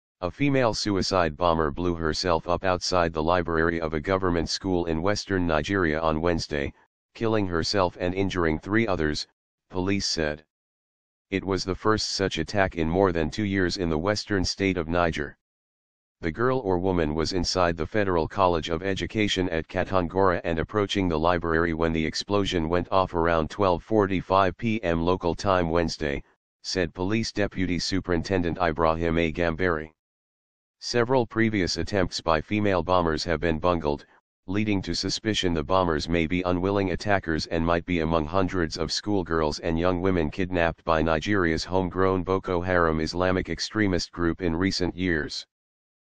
Suicide bomber detonates bomb at Nigeria's Federal College of Education
A female suicide bomber blew herself up outside the library of a government school in western Nigeria on Wednesday, killing herself and injuring three others, police said.